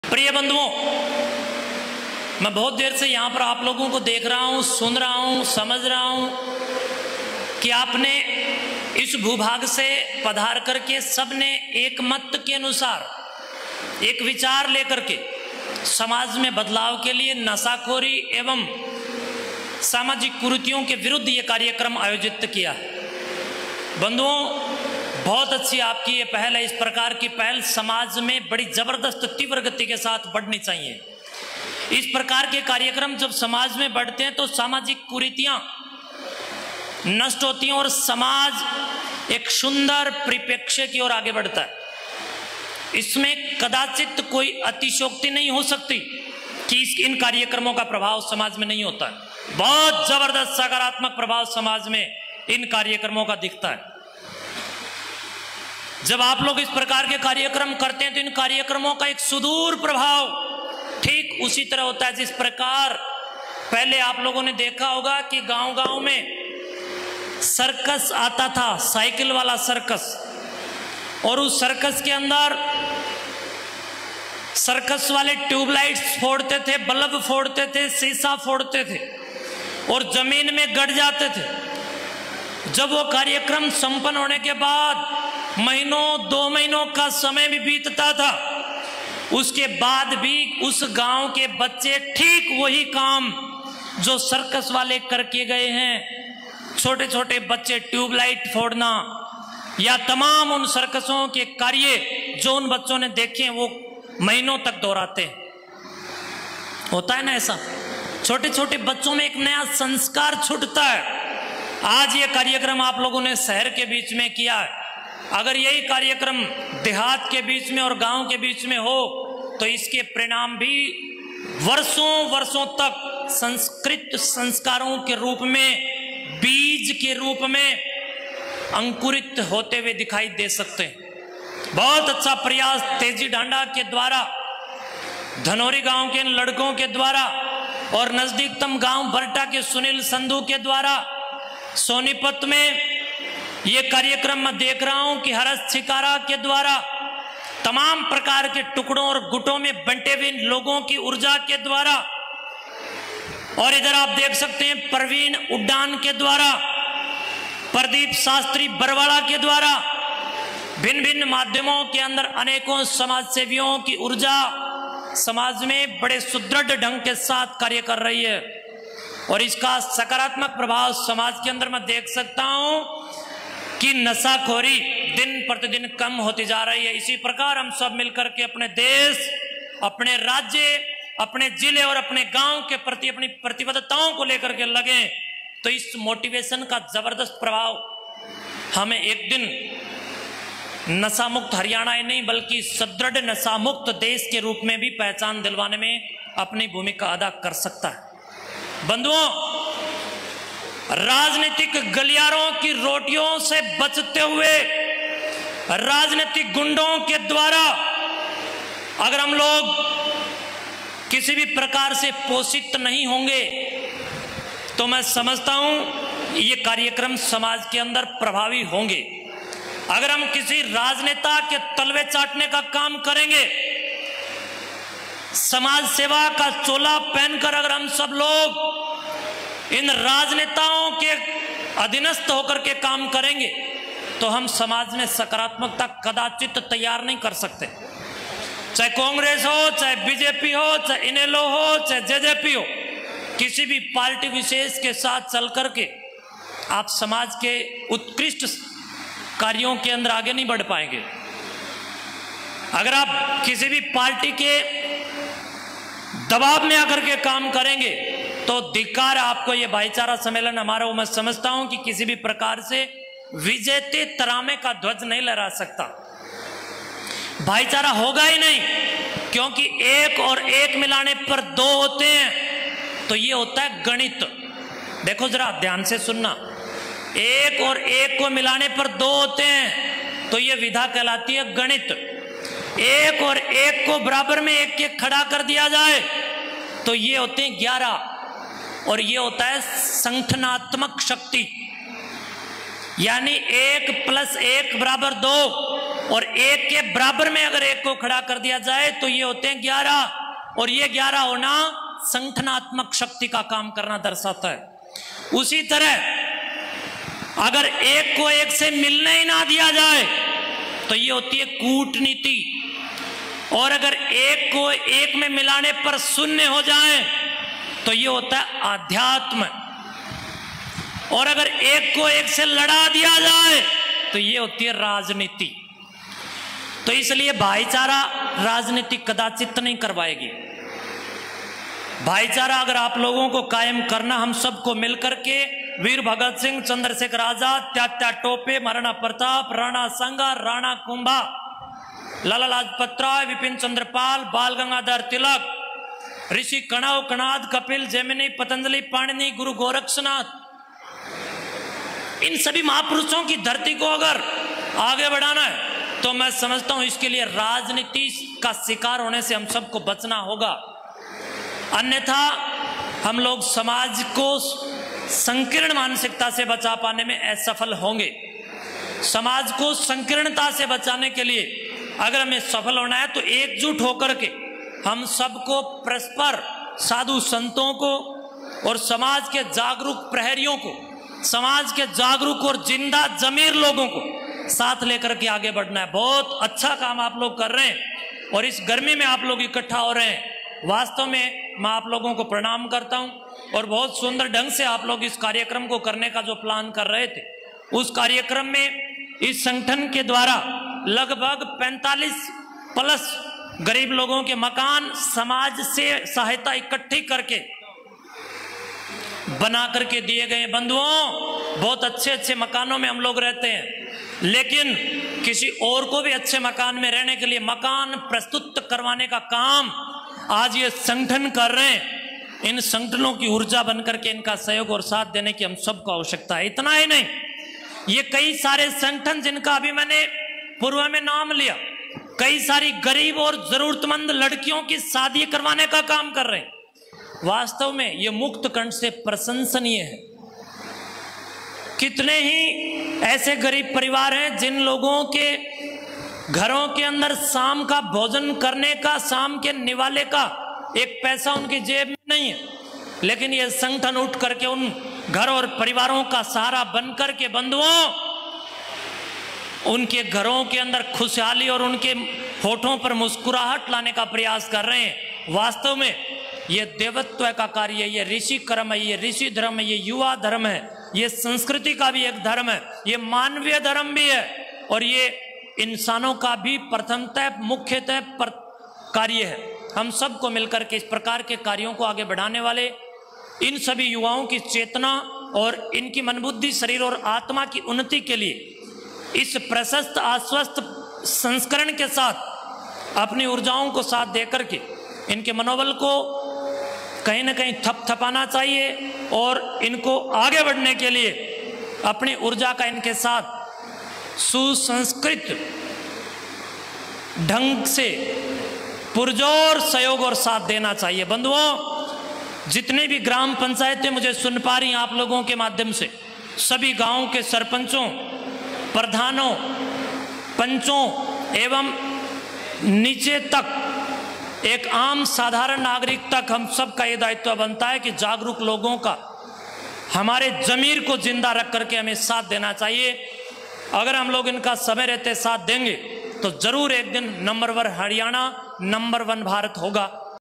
समाजसेवा का चोला पहनकर राजनेताओं के तलवे चाटने वाले कभी कामयाब नहीं हो सकते : महंत शुक्राई नाथ
-(तेजी ढांडा) Helping Hand Haryana द्वारा करवाया गया भाईचारा सम्मेलन
|Kaithal Breaking Tv|